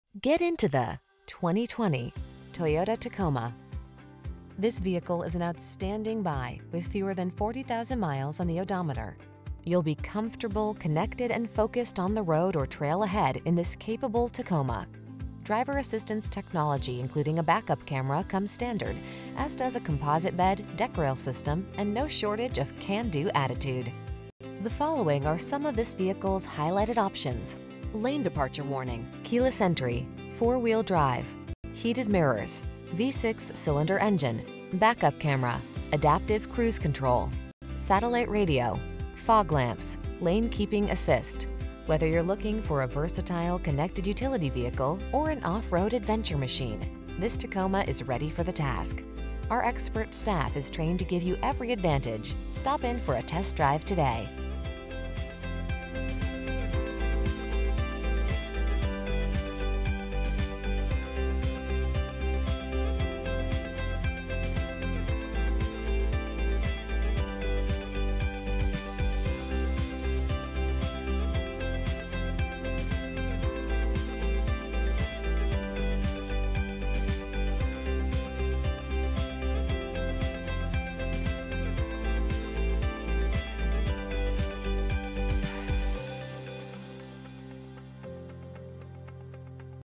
2020 Toyota Tacoma_4WD Gainesville, Westgate, Oakwood, Pleasant View, New Holland U14512A
BLACK Used 2020 Toyota Tacoma_4WD TRD Off Road available in 2407 Browns Bridge Road, Gainesville, Georgia at Jacky Jones Sales and Service. Servicing the Gainesville, Westgate, Oakwood, Pleasant View, New Holland, GA area.

Jacky Jones Sales and Service
2407 Browns Bridge Road